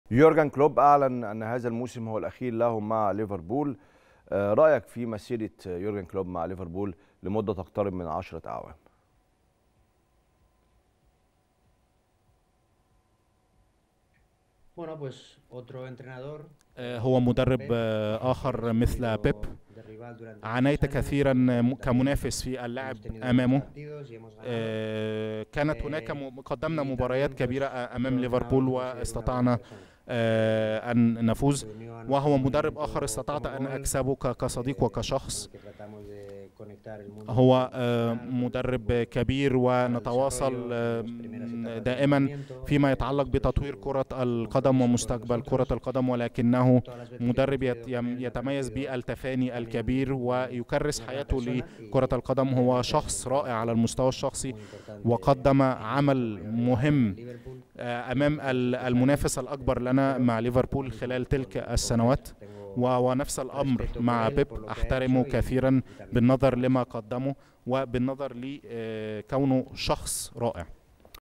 "نتواصل دائما من اجل الاستفادة 💥💥خوان ماتا بكشف لاول مرة عن علاقته بـ يورجن كلوب ⚽️👇🏻
"نتواصل دائما من اجل الاستفادة من تطوير كرة القدم"💥💥خوان ماتا بكشف لاول مرة عن علاقته بـ يورجن كلوب ⚽️👇🏻